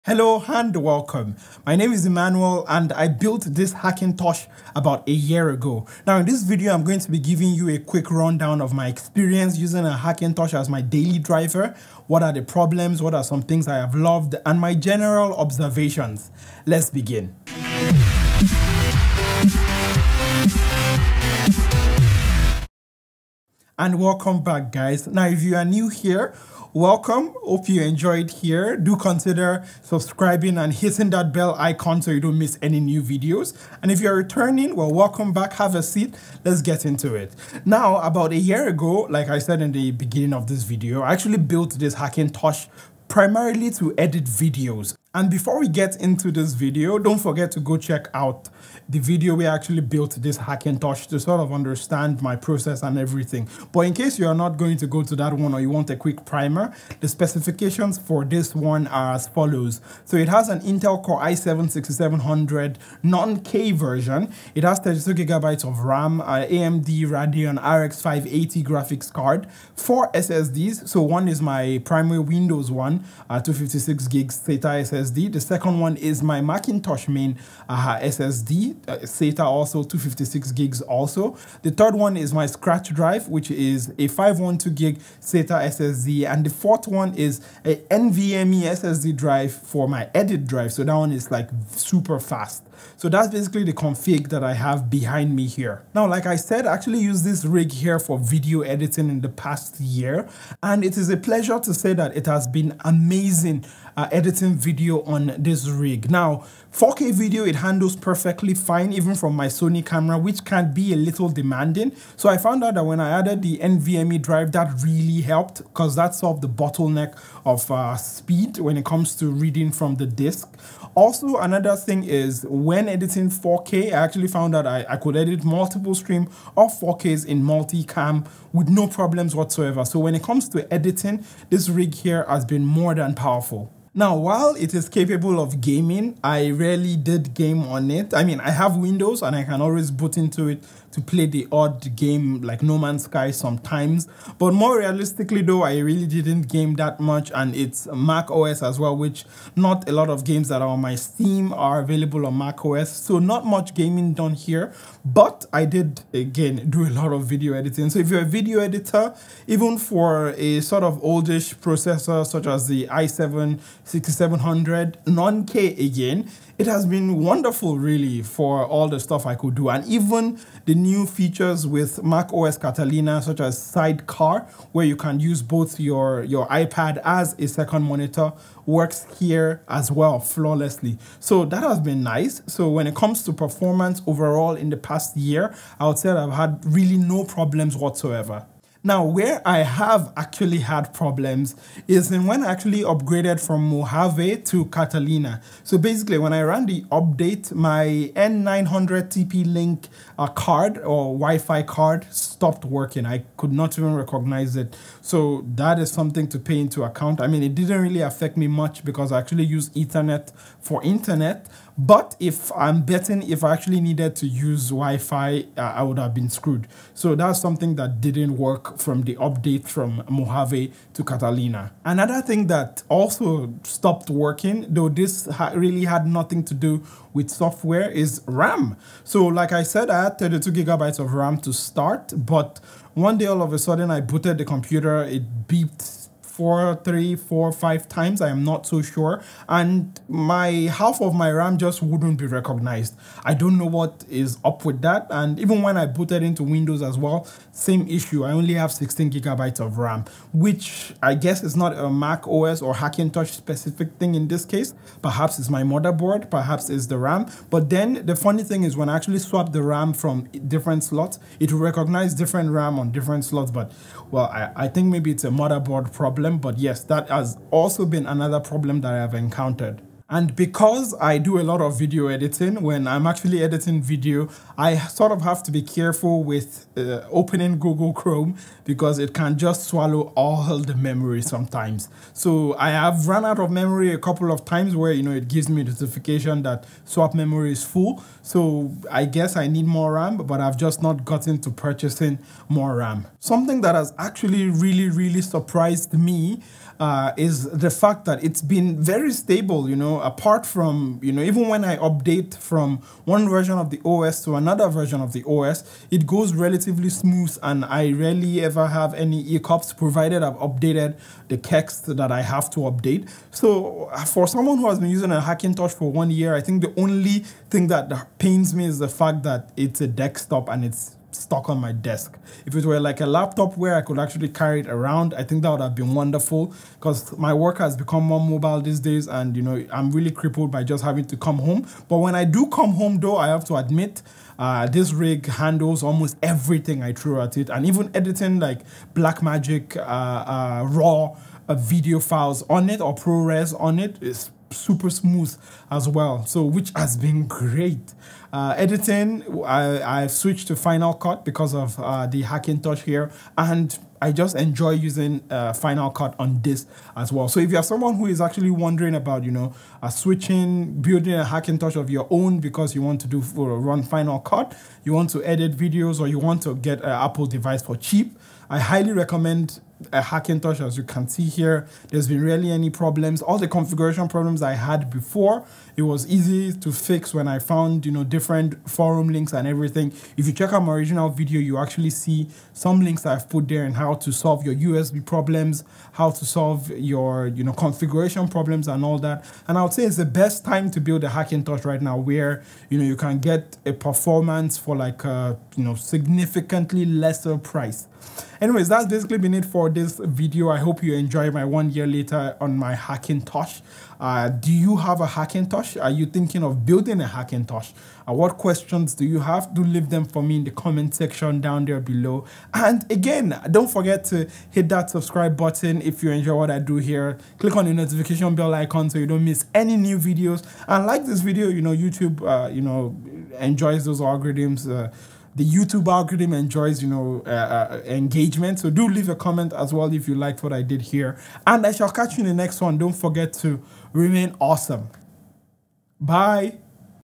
Hackintosh For Video Editing After One Year! | Is it Worth It?
It has been a little over a year since I built this Hackintosh specifically for video editing in FCPX. In that time I've edited several mini-docs, YouTube videos, and other kinds of social media content. I've thrown Multicam 4k footage, big prores raw footage and more at this rig. How has it performed with my workload and in the duration of the year? Do watch the video to find out.

Specifications
-Intel Core i7 6700 CPU
-Cooler Master 750W PSU
-Gigabyte Z170XP-SLI Motherboard
-16GB RAM
-RX580 8GB GPU

Fixing USB3 Ports on Hackintosh by generating SSDT or USBMAP.kext